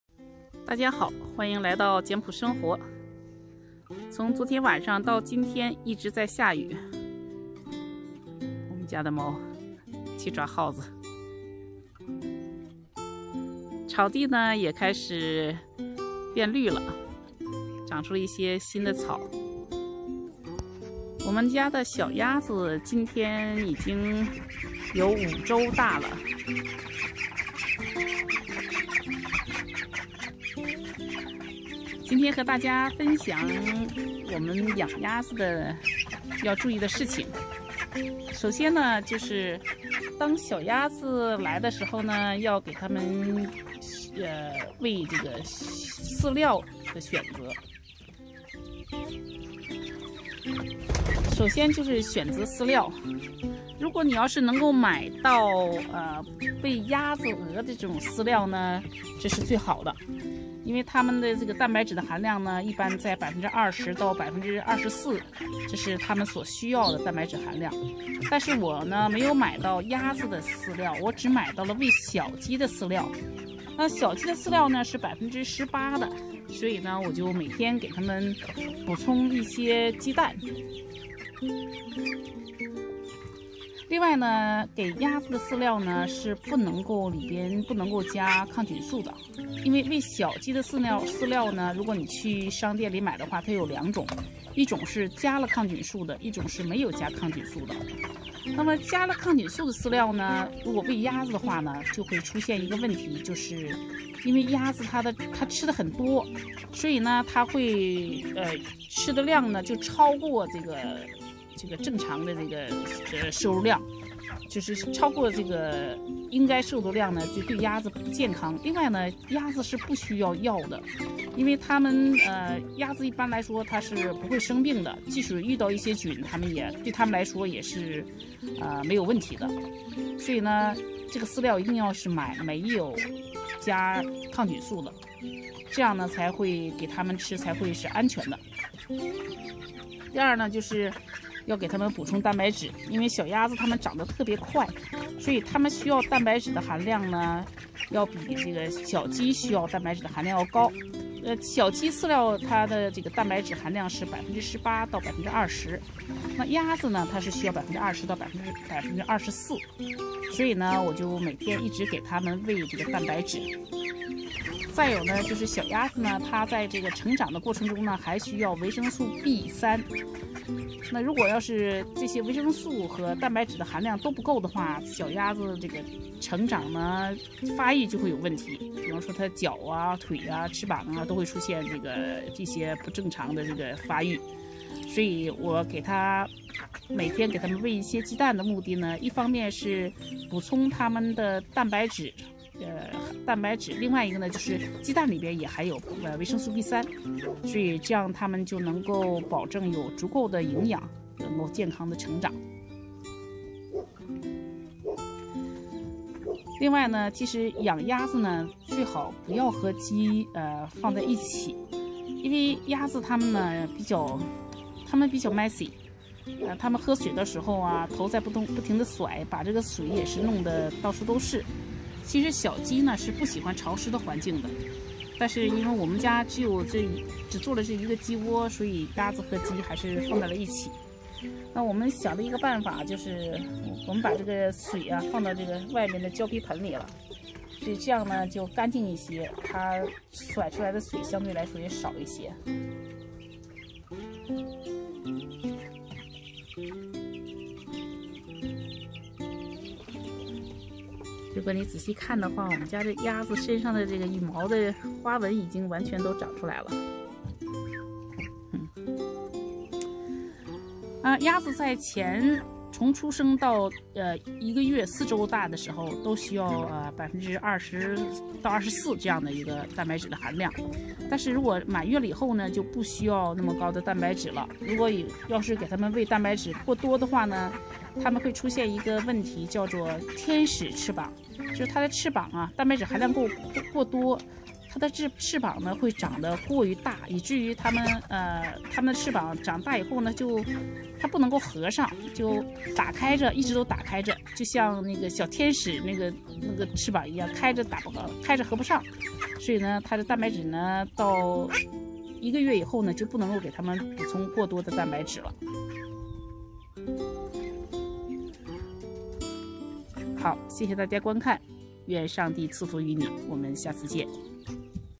养鸭：为0-4周大的小鸭选择饲料 （蛋白质含量，维生素B3，抗菌素）
Duckling Feed